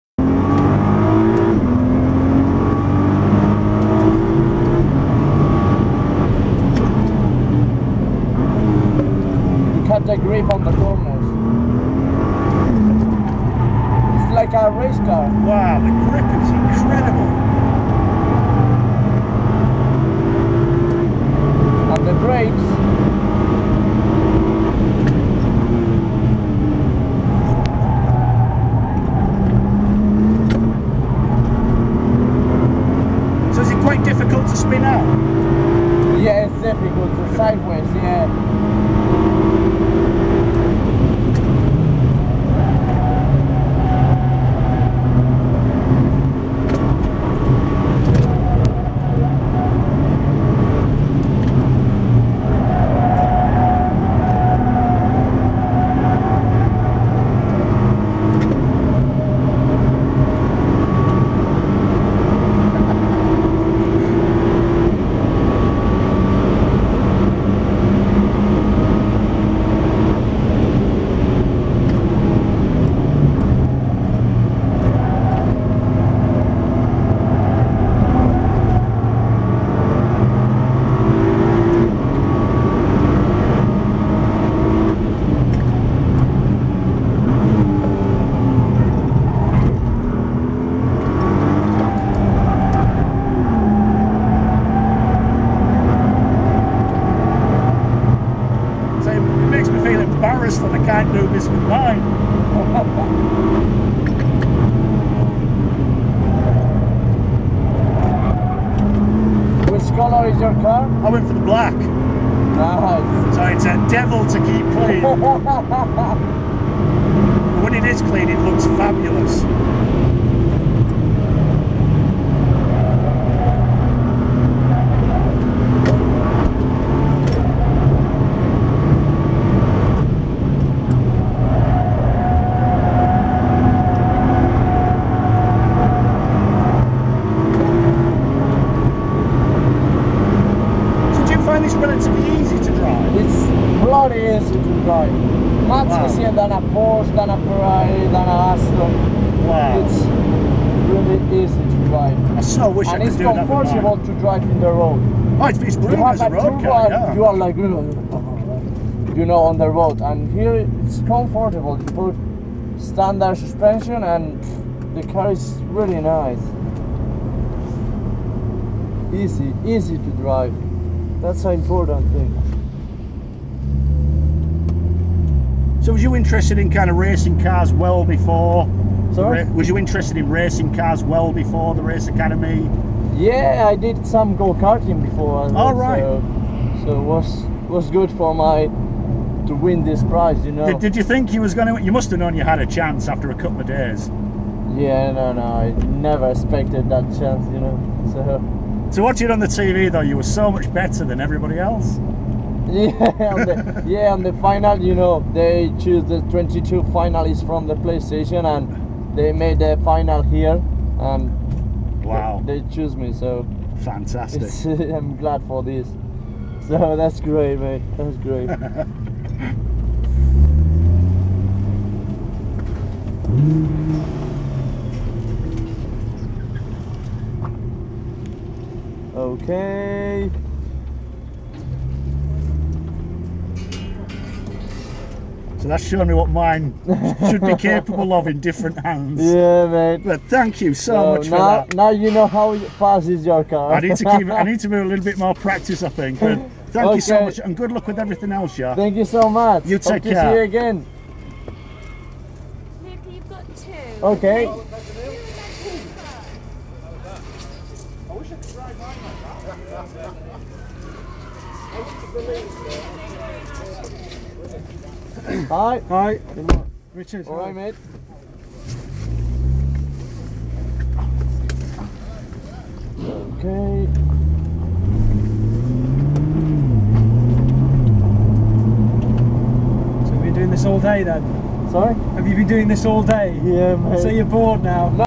GT ACADEMY WINNER Lucas Ordoñez Nissan GT-R fast ride at Silverstone
High speed passenger ride at Silverstone Southern circuit in the Nissan Race Academy event. Nissan GT Academy Playstation winner Lucas Ordoñez shows  the Nissan GT-R has no limits on track...